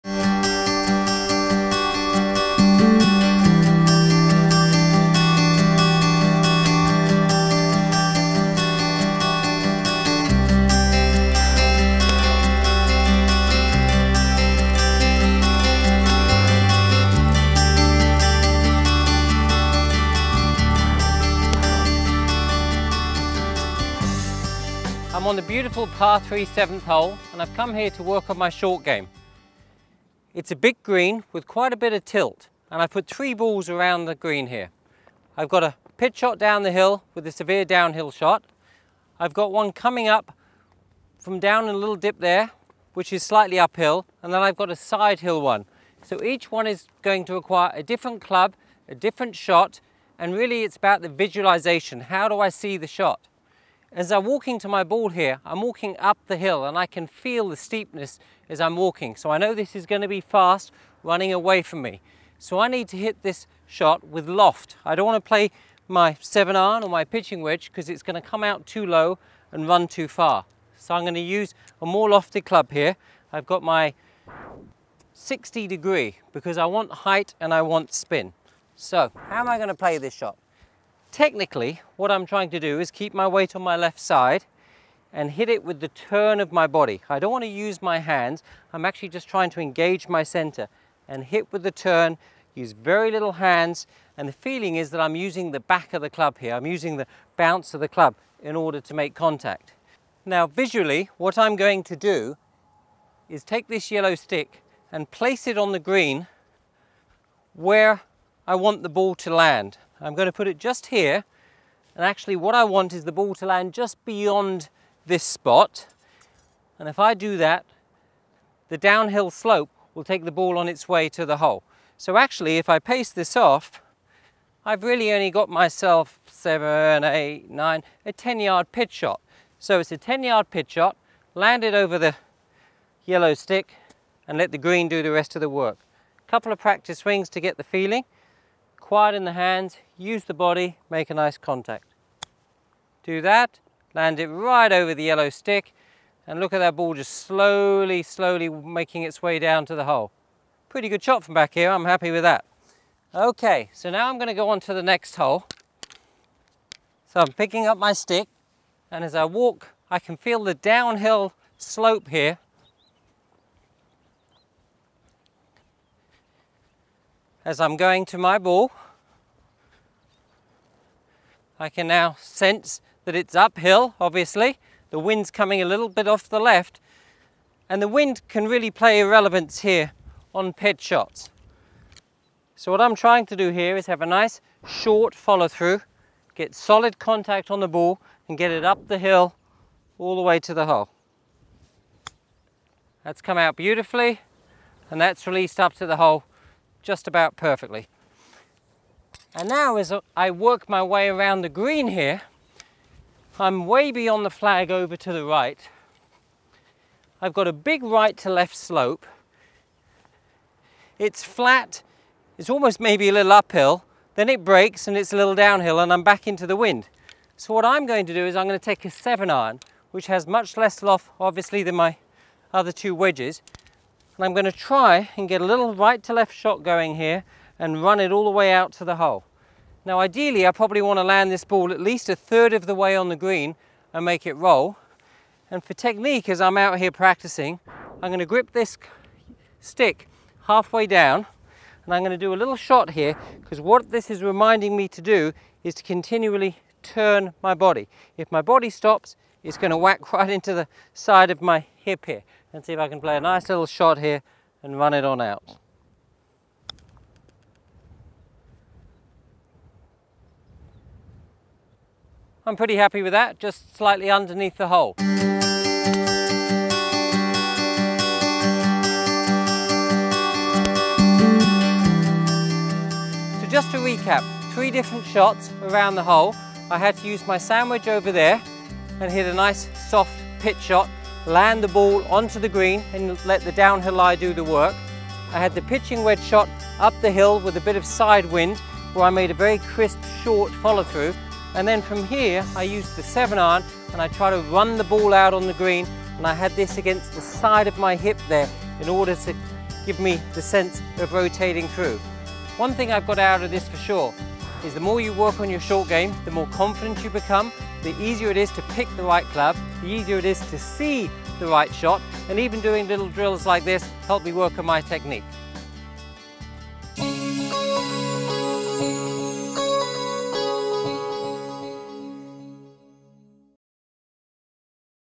GW Instruction: Play Like a Pro - Lesson 19 - Pitching, Visualisation
Learn how to improve your golf game with Professional Golf Coach Mitchell Spearman